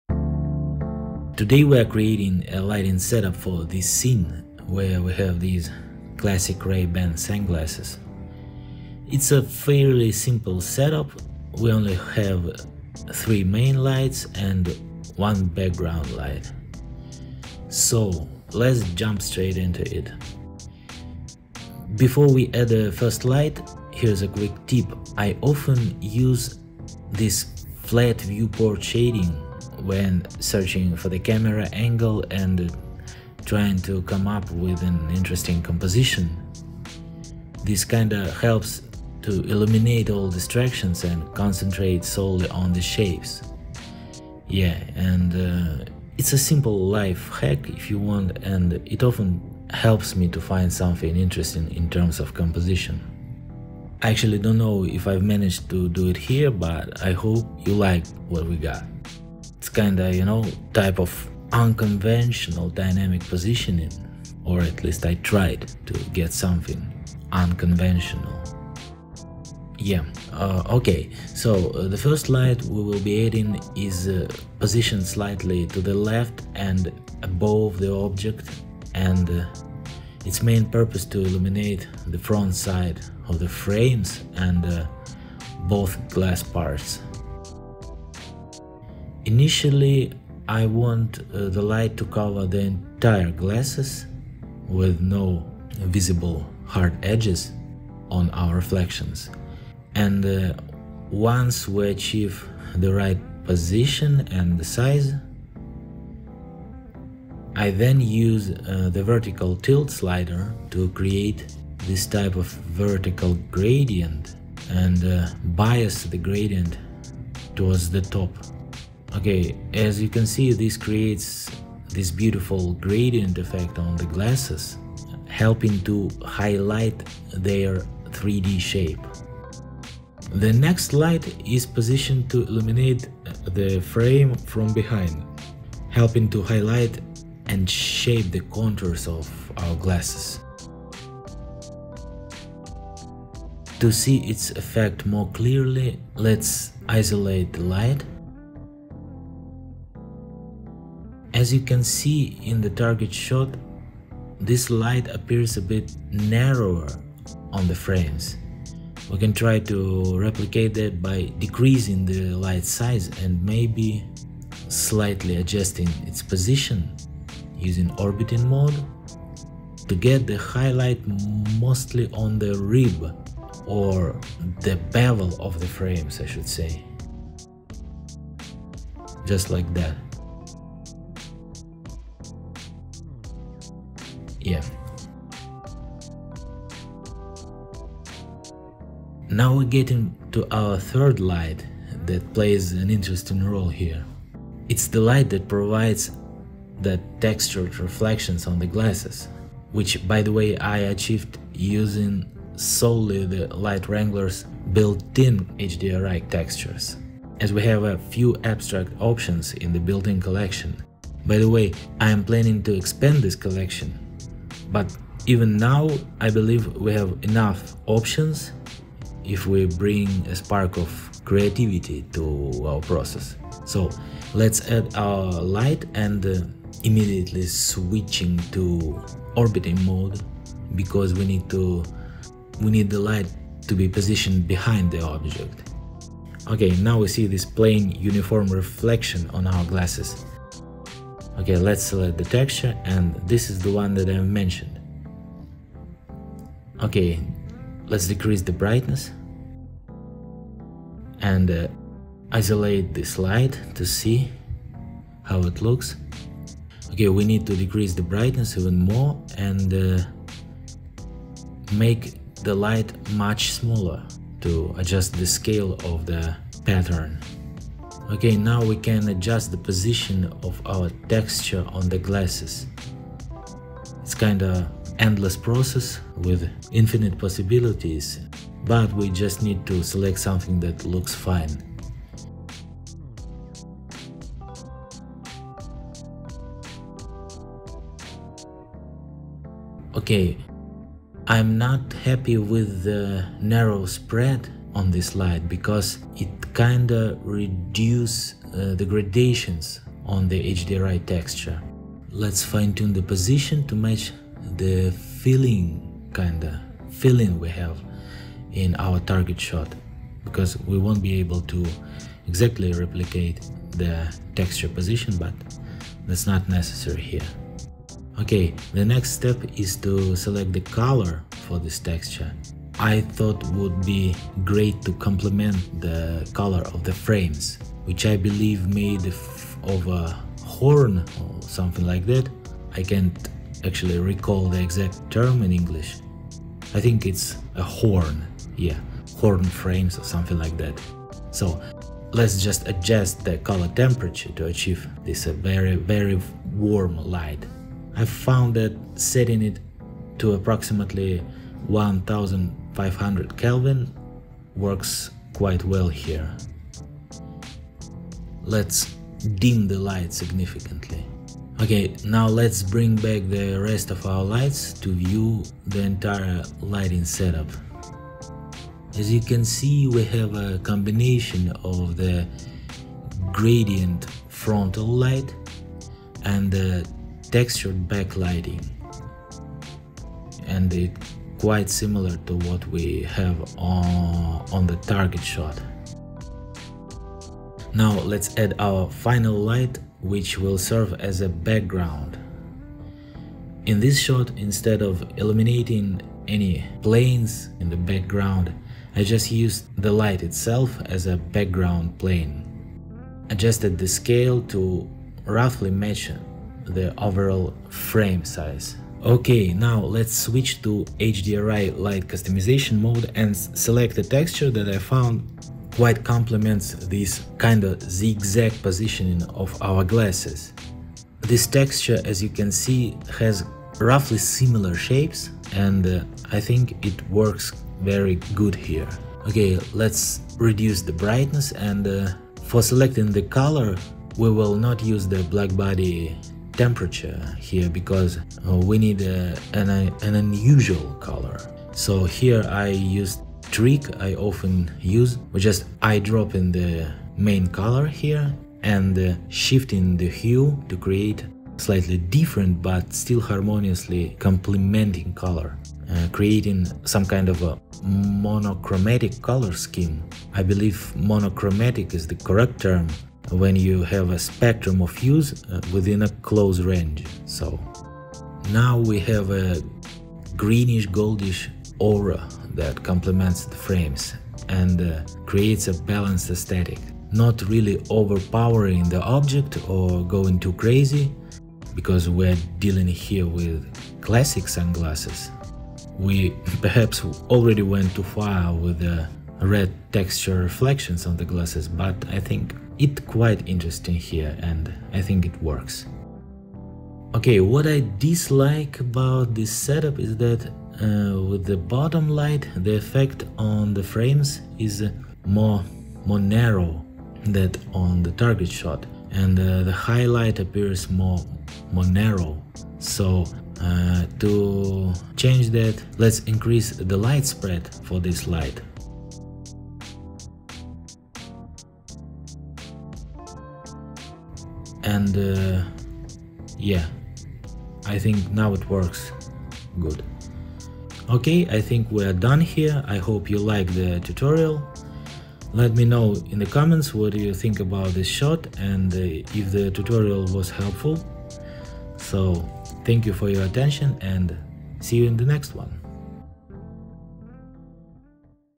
Blender Tutorial: Stylish Ray-Ban Sunglasses Lighting with Light Wrangler
🌟 Elevate Your Ray-Ban Sunglasses Product Shots with Light Wrangler! 🌟

In this Blender tutorial, we'll walk you through the process of creating a stylish and unconventional lighting setup for a pair of classic Ray-Ban sunglasses using the Light Wrangler add-on. Discover how to use a combination of gradient frontal lighting, textured backlighting, and a complementary background light to create a visually striking and balanced composition.

🔑 What You'll Learn:

Positioning lights to illuminate the front and back of the sunglasses
Using built-in HDRI textures to create interesting reflections
Adjusting light colors and temperatures to complement the frames
Achieving a balanced and harmonious aesthetic with a analogous color scheme

Whether you're a beginner looking to improve your lighting skills or an experienced artist seeking to experiment with unconventional setups, this tutorial has something for you.

With Light Wrangler, you'll be able to create a unique and eye-catching lighting setup that showcases the beauty of these classic Ray-Ban sunglasses.

🔧 Tools Used:

Blender 4.1.1
Light Wrangler Add-on 1.9.6
🚀 Expand Your Lighting Possibilities!
It's packed with powerful features and tools to help you create stunning, professional-grade lighting in your scenes.

I'd love to hear your thoughts on this unconventional lighting setup for the Ray-Ban sunglasses.